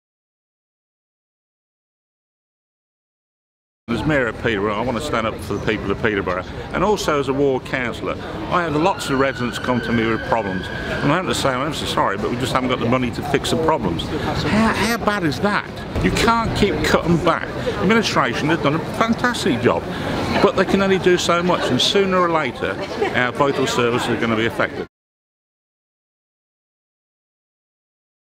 Stand Up For Peterborough - The Mayor John Fox
'Stand Up For Peterborough - Protect Our Vital Services' is a campaign  being led by the council to lobby government for fairer funding for Peterborough.

Find out more about the campaign and pledge your support on the council